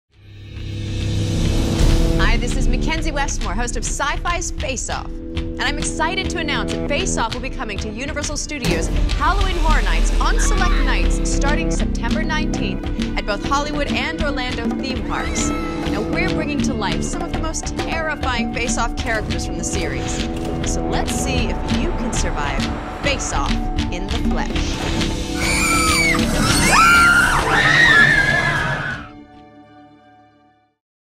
Face Off Comes to Halloween Horror Nights 24
Syfy's Face Off comes to Halloween Horror Nights 24 as the first Scare Zone announced for 2014!